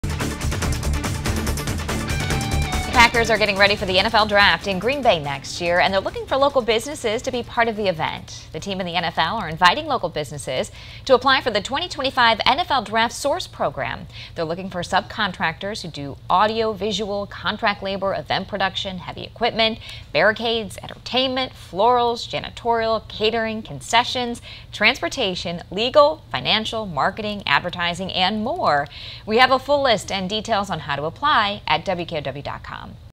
Wisconsin businesses encouraged to apply for involvement in 2025 NFL Draft in Green Bay
The Packers and the NFL are looking for Wisconsin businesses to be part of the 2025 NFL Draft in Green Bay.